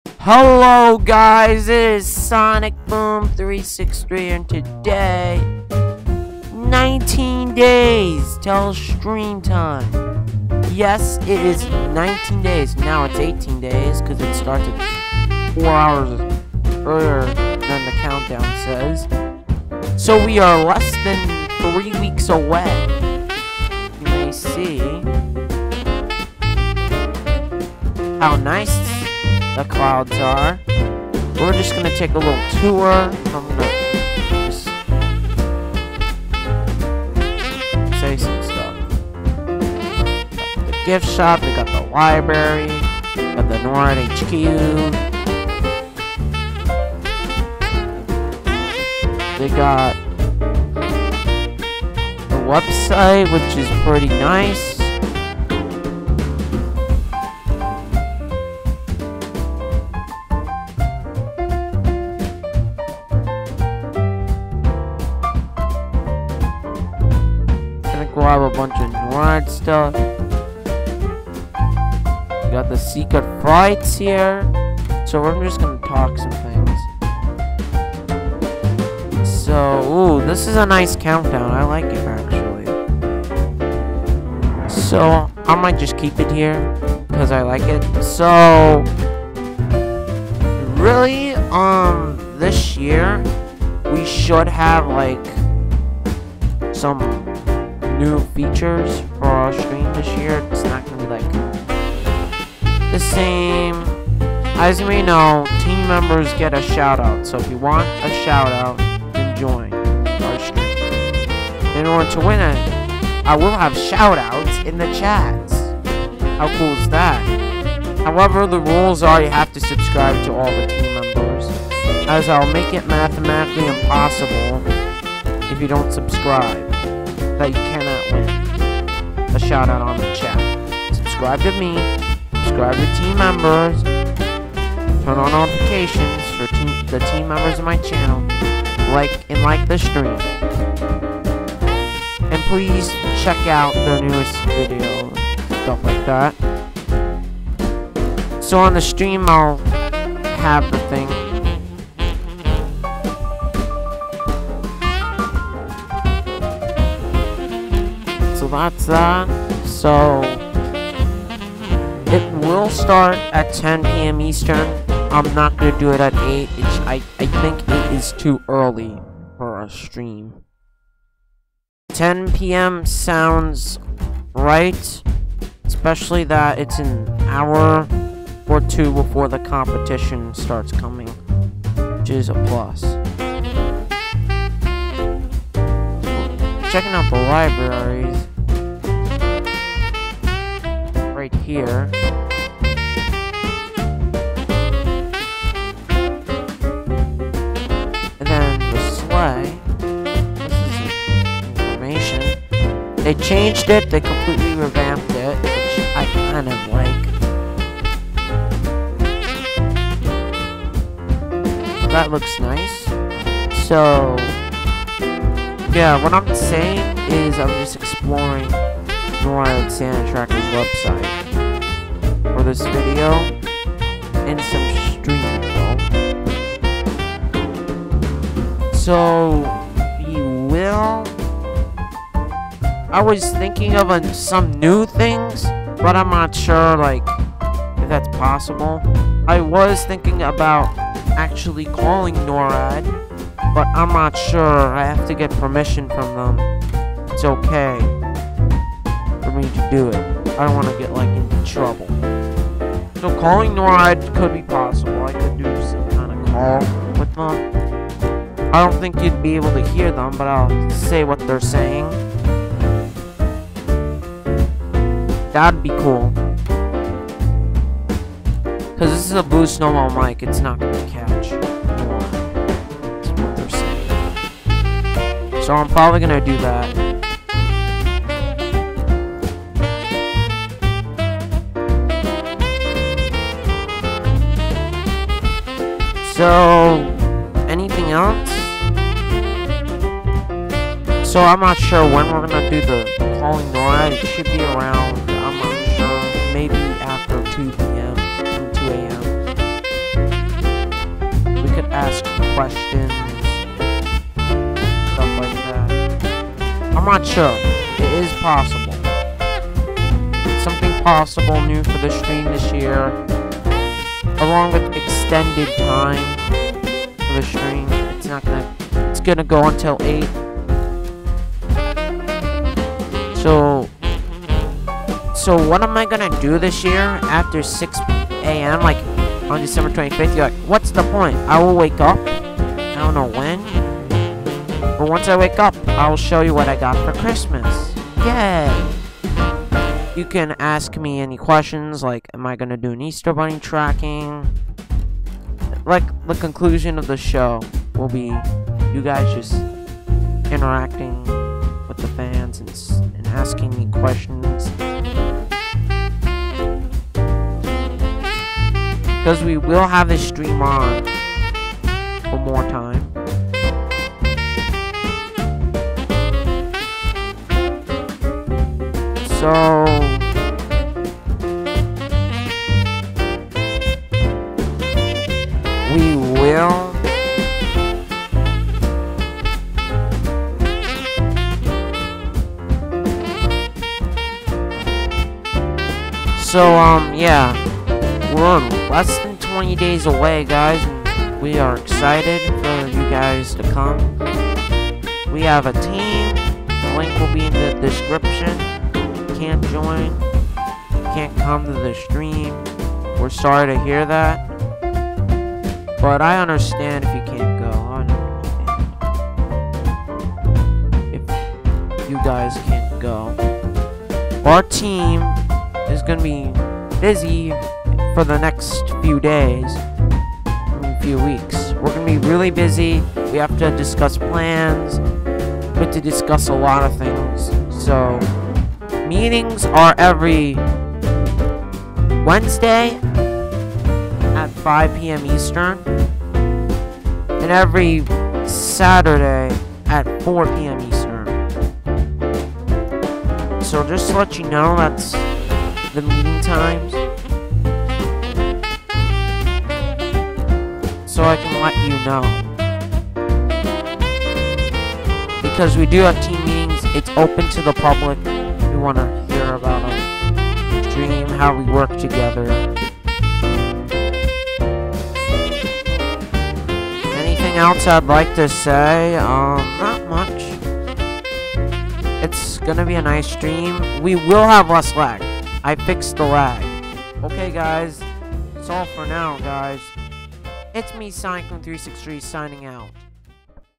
Visiting NORAD Santa Tracker! New Info on 2017 NORAD Santa Tracker Live Stream
New Info on 2017 NORAD Santa Tracker Live Stream?
19 Days to the Stream... Right? Yeah so we have improtant information INCLUDING shoutouts on the Santa Tracker Stream, the biggest event on our channel, it is a recurring event too! But we will go into further depth about the rules for a shoutout (It will be Subscribe to the Team Members, Like the Stream, and Subscribe to me, and stay active kind of thing) in a future video, and we have another potential surprise for the stream, but you will have to watch the video for that!
Sorry if the videos have gone a little repetitive, i will have a 2k18 video out later today, along with another NORAD Santa Tracker Video.

Add me on G+ Fruck you guns and swords, this video is not targeted for kids. FRUCK!